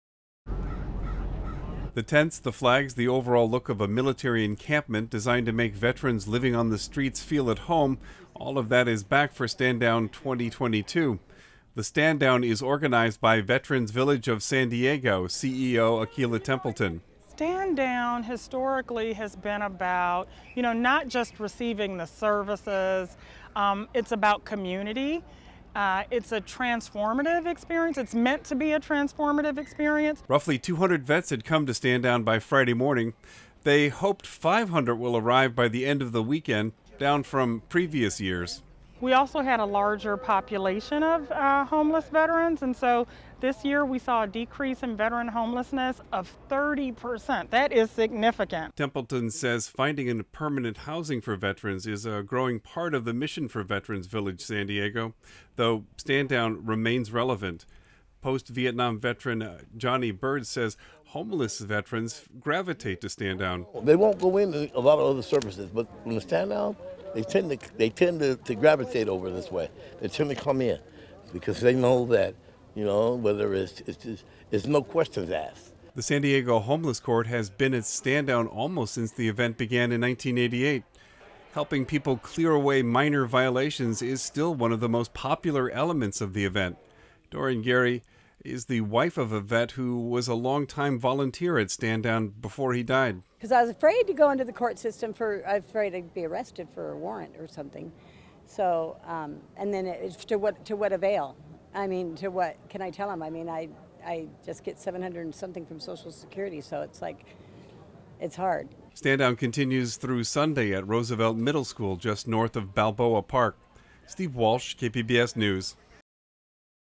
Stand Down event for homeless veterans returns in full
Stand Down is back in full swing this weekend, after being shortened by the pandemic for two years. KPBS Military Reporter Steve Walsh says organizers believe the effort to help homeless vets is still relevant, despite changes in the homeless population.